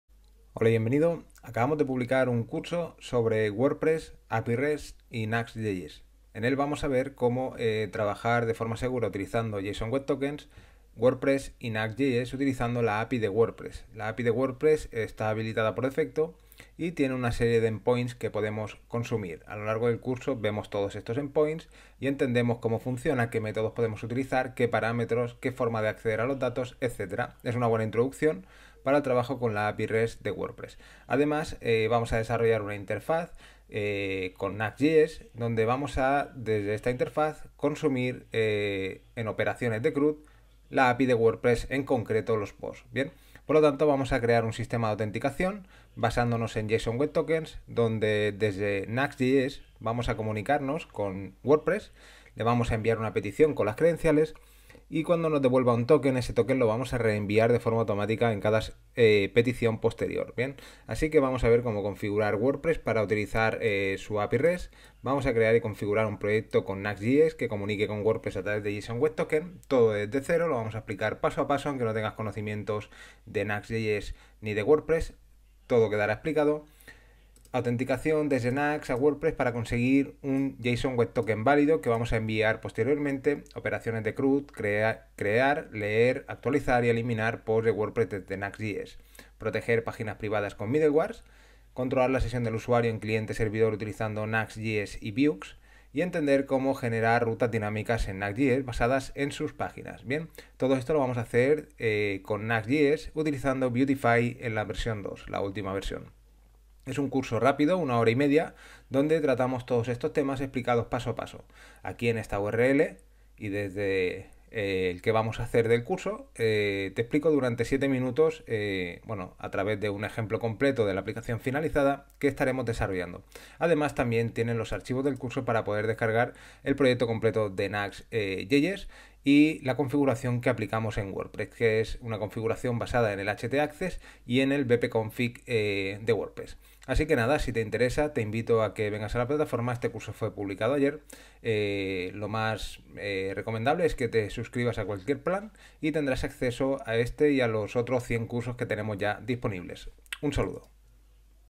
Curso WordPres API REST y Nuxt.js con JSON WEB TOKENS
Un curso rápido directo al grano para aprender a configurar este tipo de proyectos paso a paso y de forma correcta, WordPress REST API + Nuxt.js con JWT.